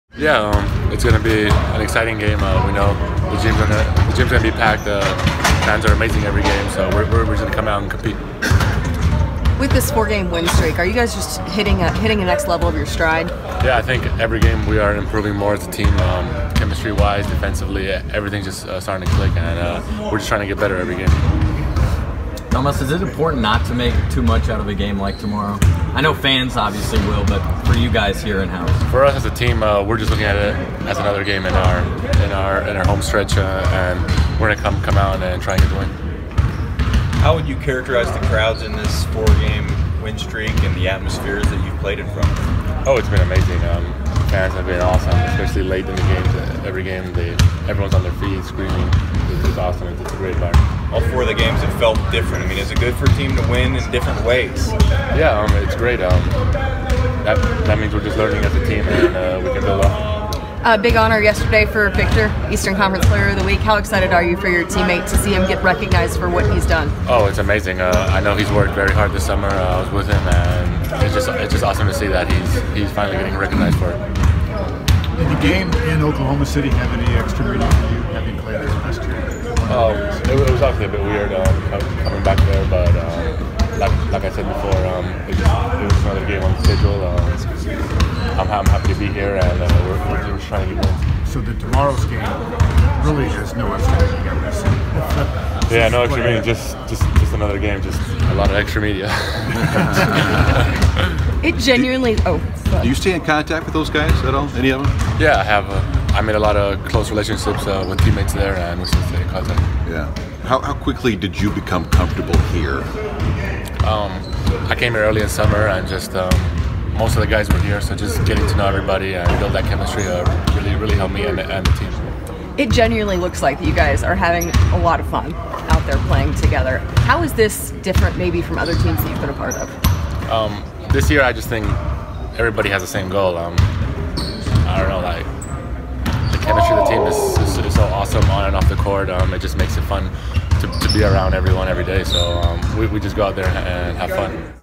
Domantas Sabonis ahead of facing his former team, OKC
Pacers center Domantas Sabonis spent his rookie season in Oklahoma City, and is now thriving in Indy. He says playing the Thunder will be just another game.
Scott Agness runs VigilantSports.com covering the Pacers.